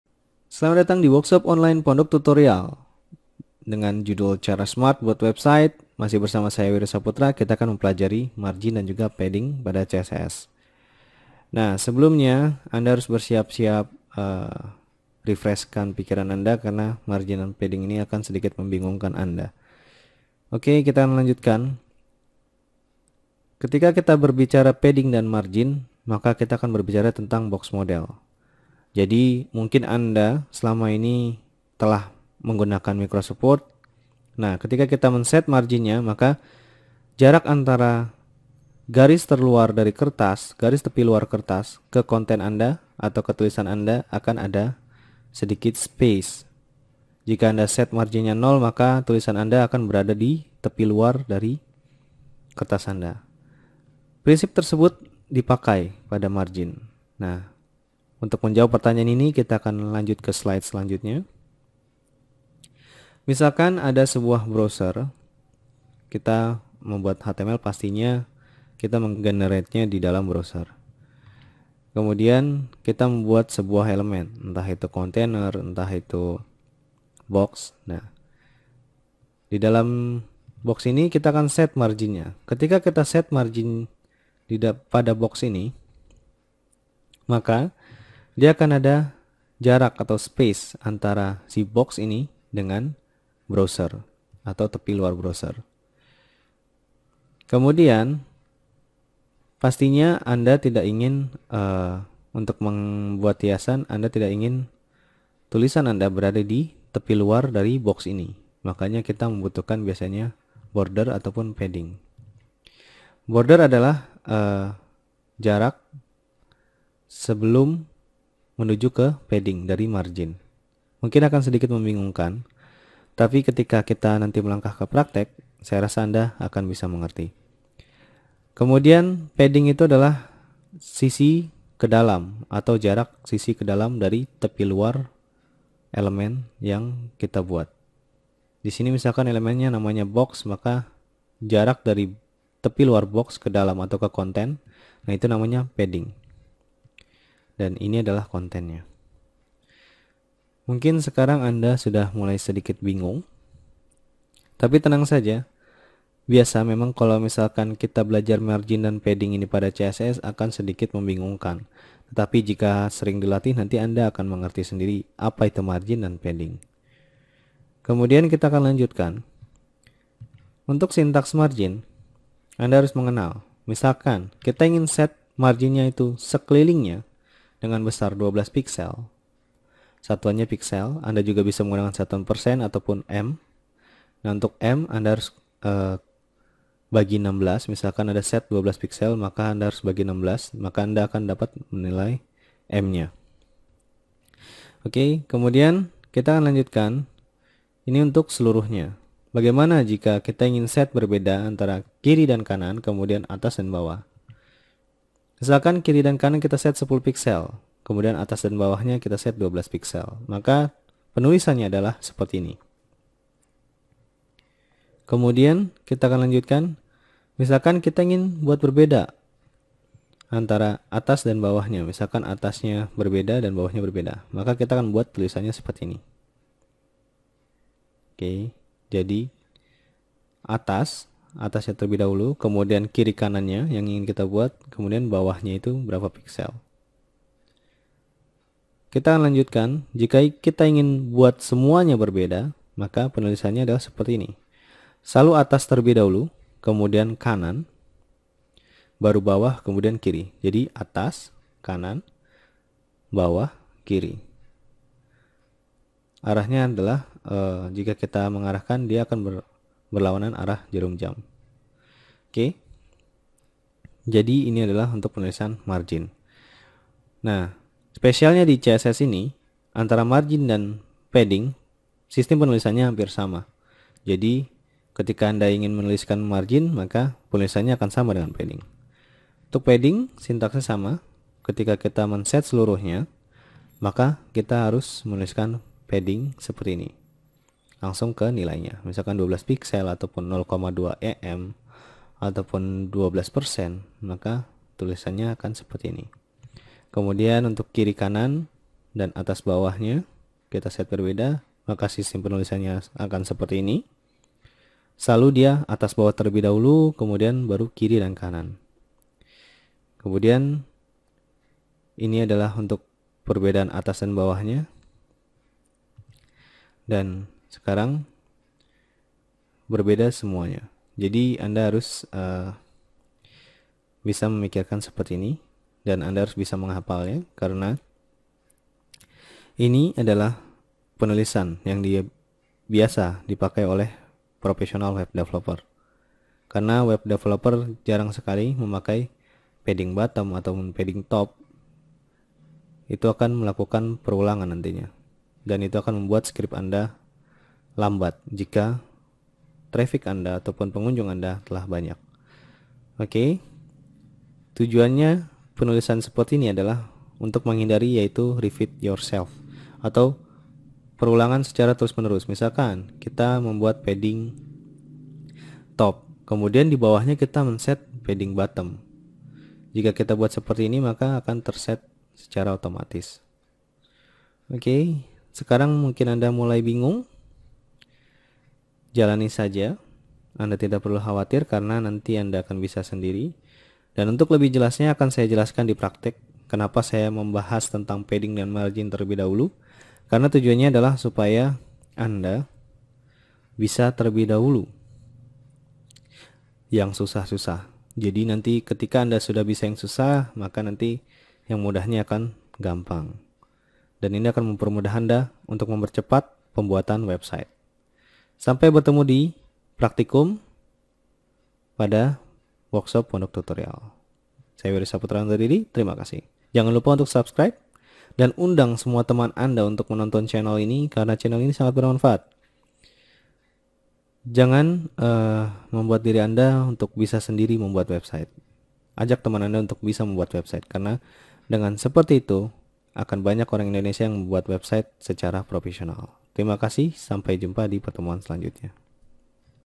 Kursus Buat Web Gratis:Margin Dan Padding
Pada tutorial kali ini kami membuat sebuah workshop Cara smart buat website dimana anda akan kami berikan tutorial simple bagaimana caranya membuat website dengan cepat namun elegant.